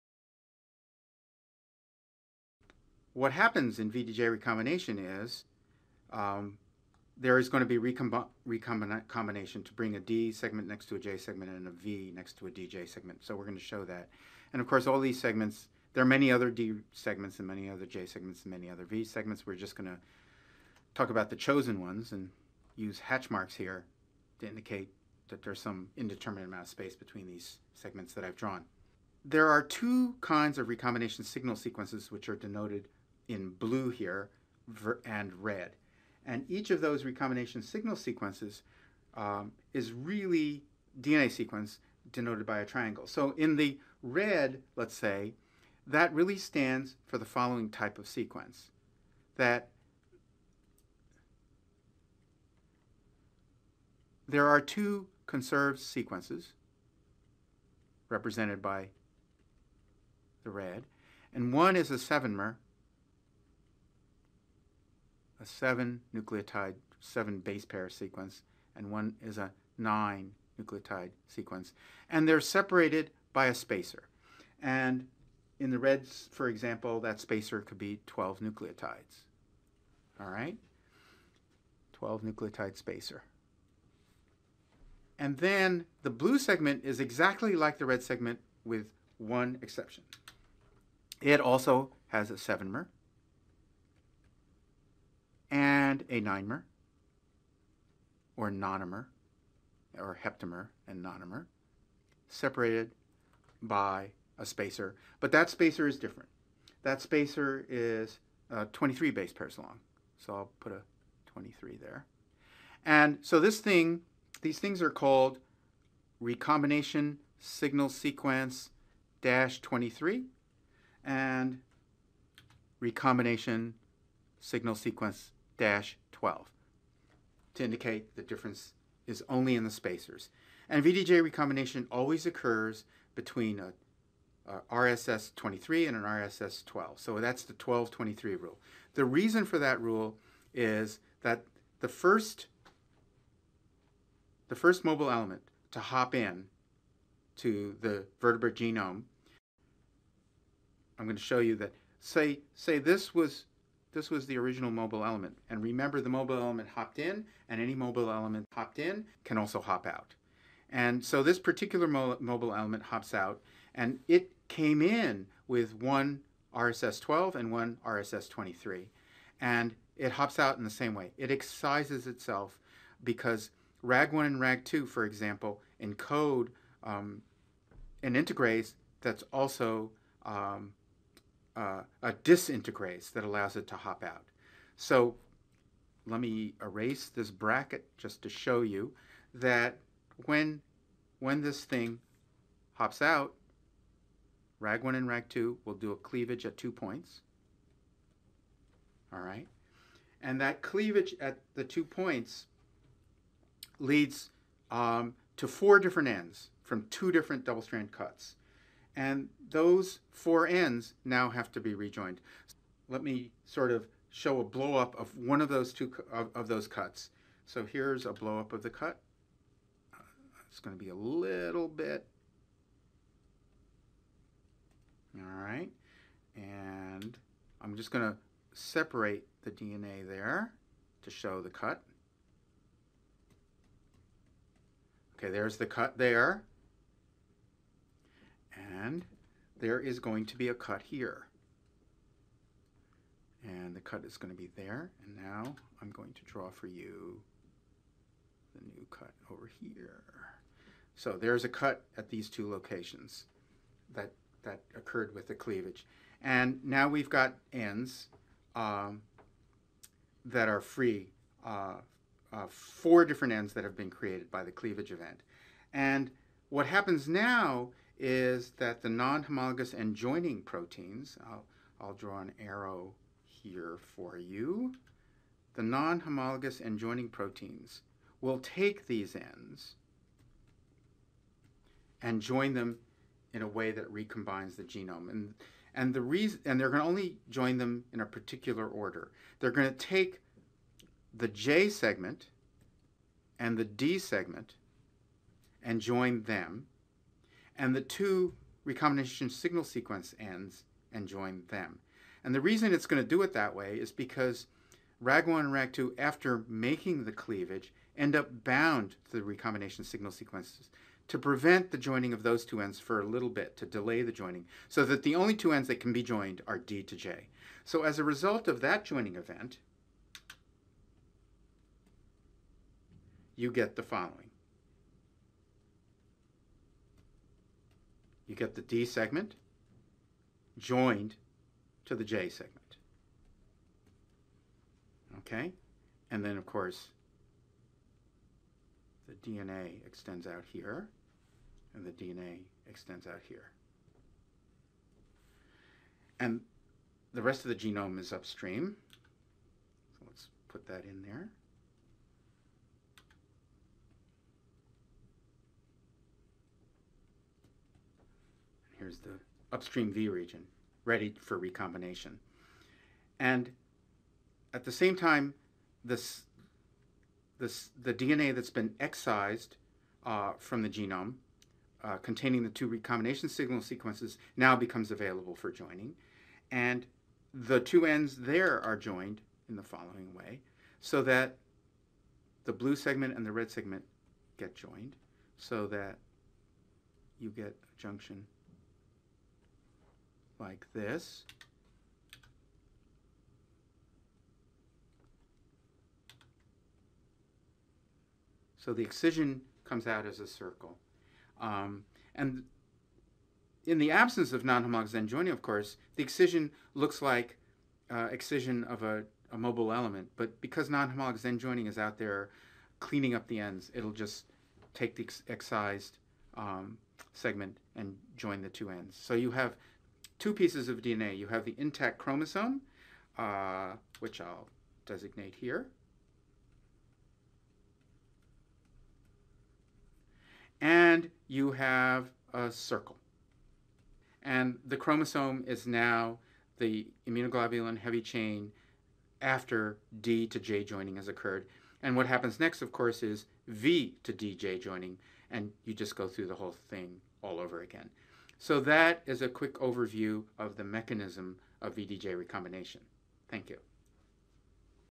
V(D)J Recombination: Mechanism
V(D)J recombination is the mechanism of somatic recombination that occurs only in developing lymphocytes during the early stages of T and B cell maturation. It results in the highly diverse repertoire of antibodies/immunoglobulins and T cell receptors found in B cells and T cells, respectively.